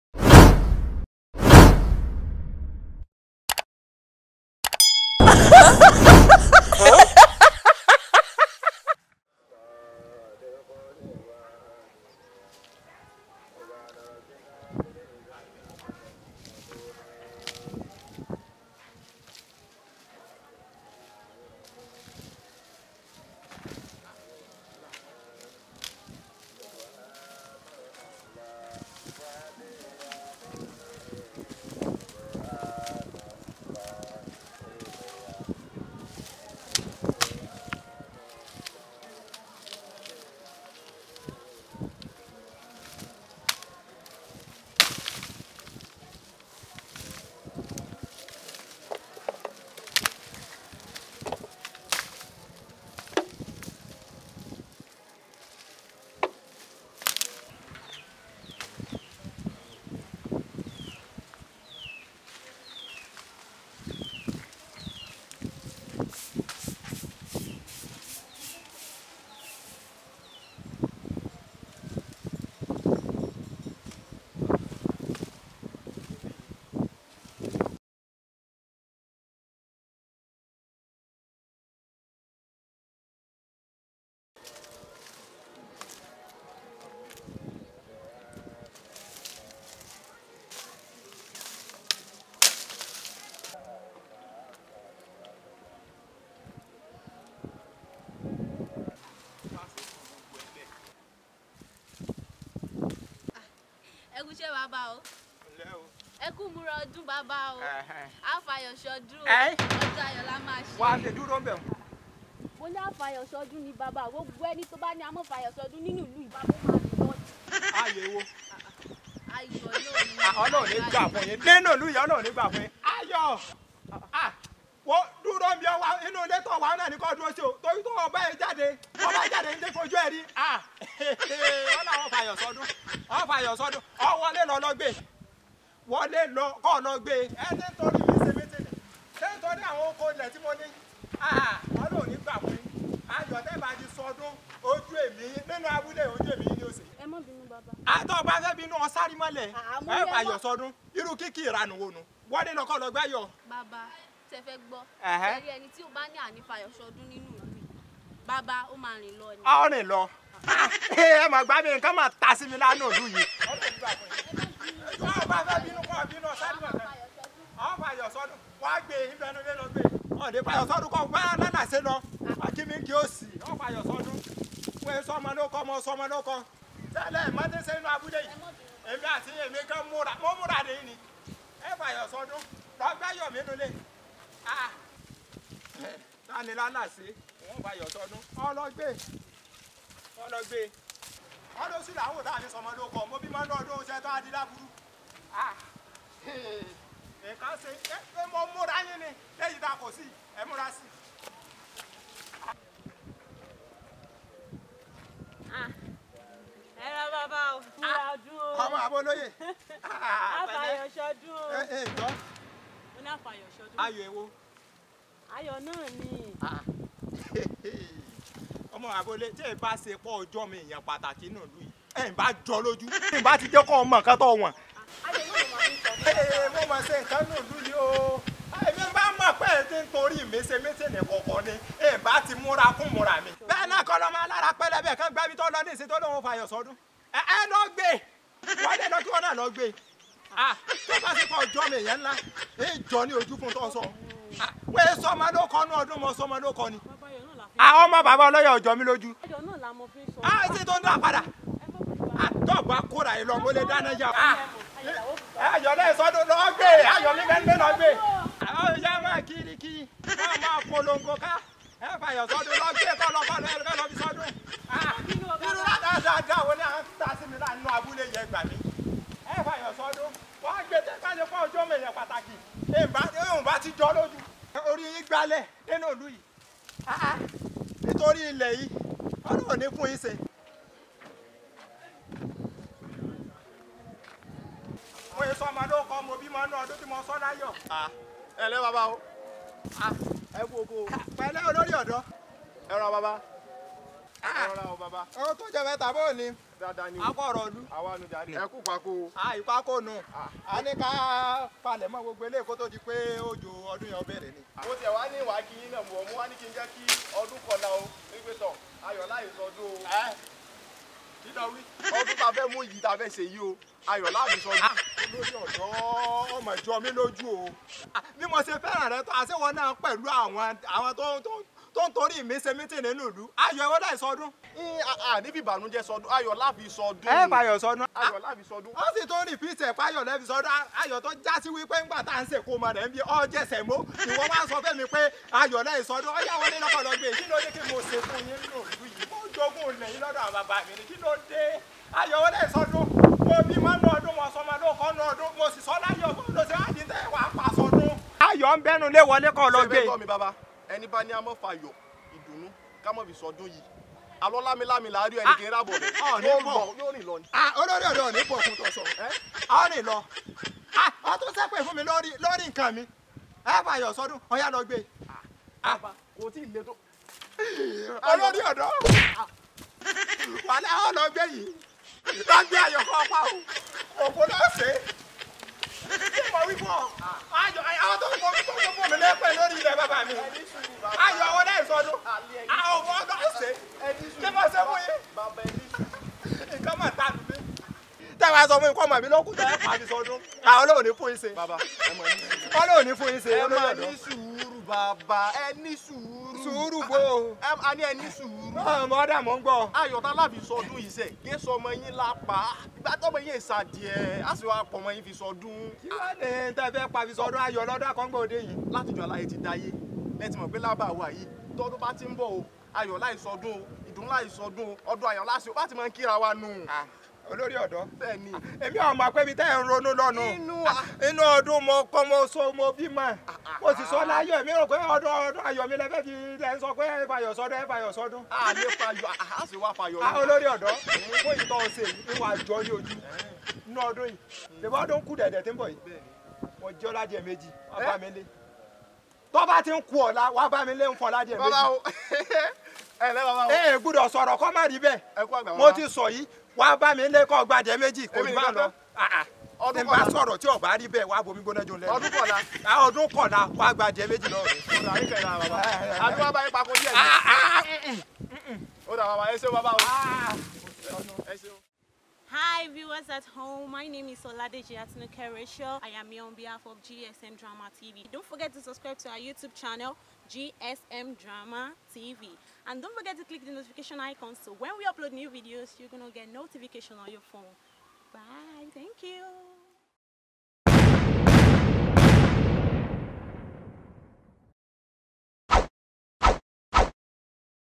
GSM TV Series: A O Fi Ayo Se Odun (Happy Easter)
This very superfluous comedy video is to appreciate our fans and lovely viewers both home and abroad for your great supports. We'll all celebrate this Easter with great joy, peace and abundant blessing.

As the saying, A o fi Ayo Se Odun (We will have a joyous celebration). This old man thought by this Yoruba prayer, some villagers wish to take the life of his son named "Ayo" (Joy).

Seeing is believing.. Watch and laugh away the sorrow of COVID-19. Better Dey Come!